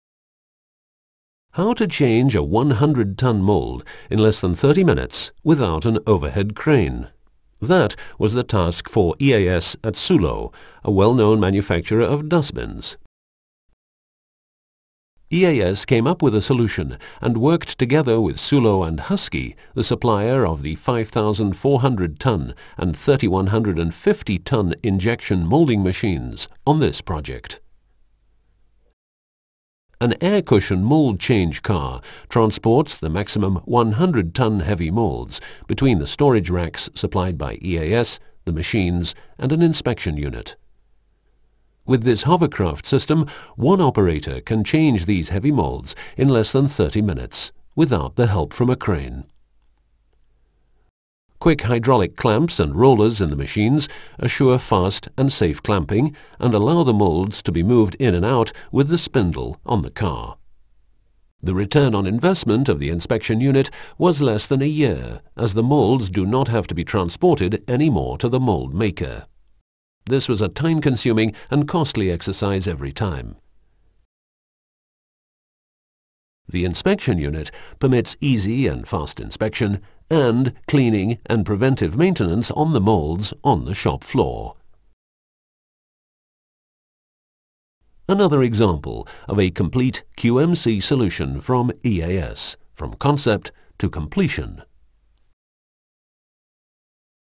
Инженерное решение компании производителя мусорных контейнеров
Литьевую форму весом в 110 тонн, обслуживает пневматический стол для транспортировки и установка для разборки.